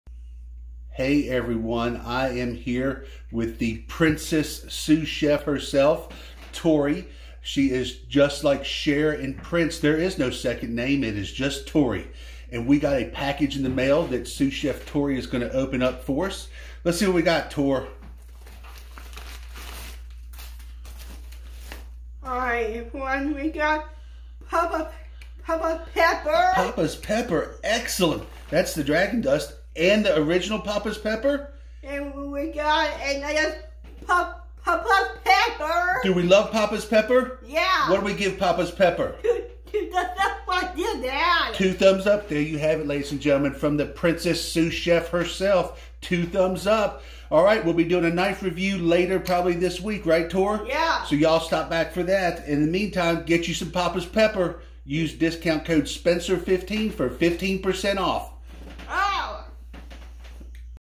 The Princess Sous Chef gets a package!
The Princess Sous Chef gets a special delivery of the worlds best premium spice blend, Papa's Pepper!